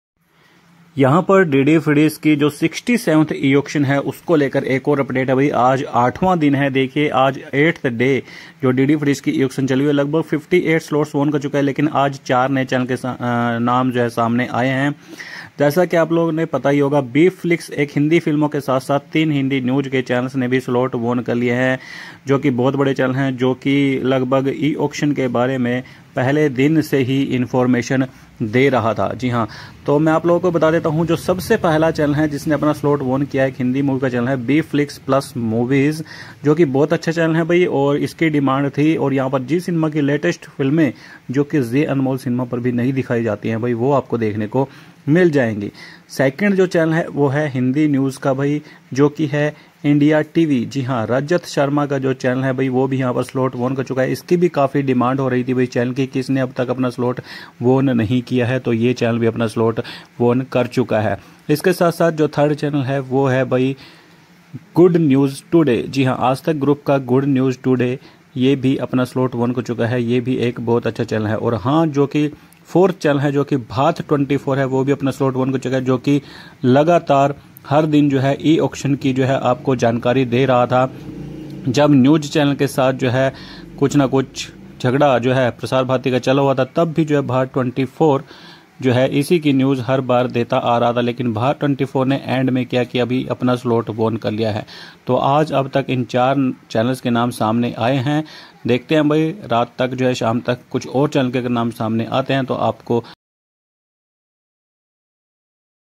Bflix Movies के साथ - साथ 3 हिंदी News चैनल्स ने जीत लिए Slots | DD Free Dish 67th E-Auction | Day 8
Bunick Talks

Thanks...